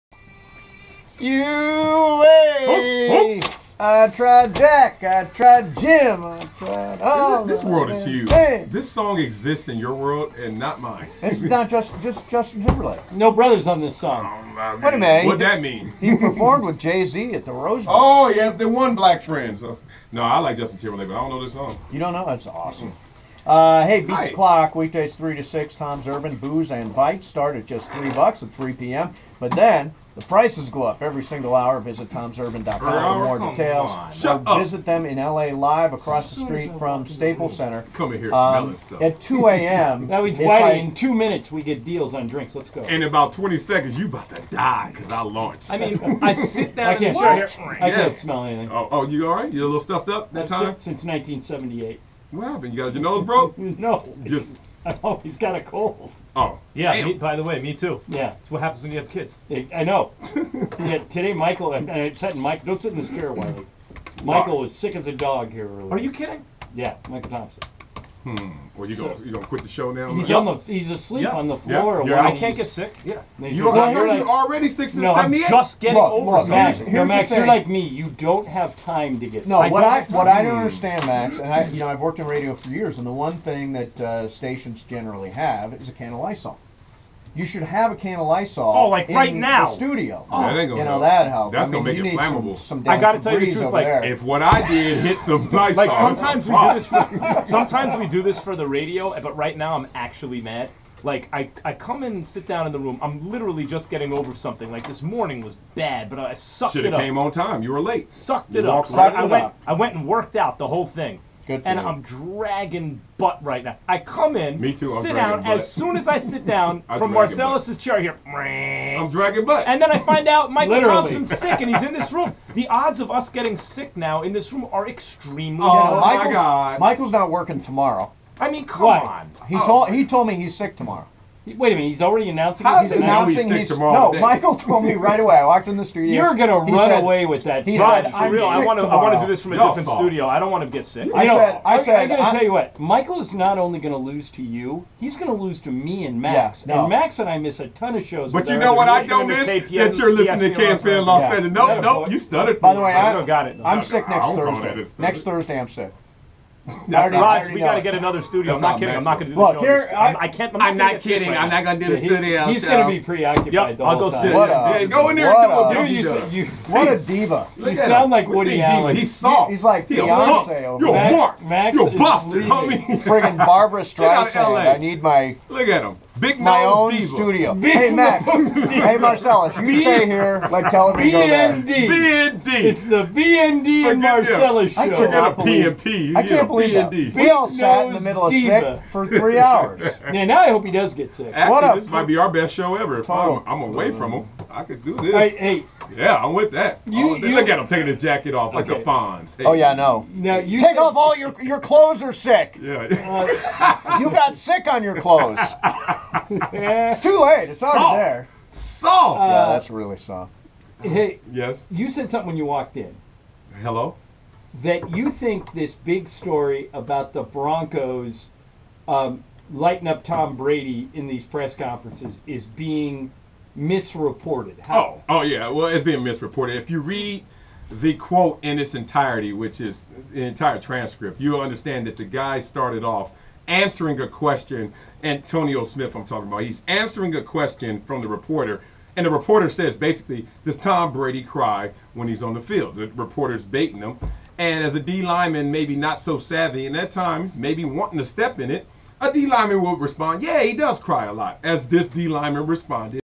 Storming out of the Studio!
Our very own Max Kellerman storms out of the studio today!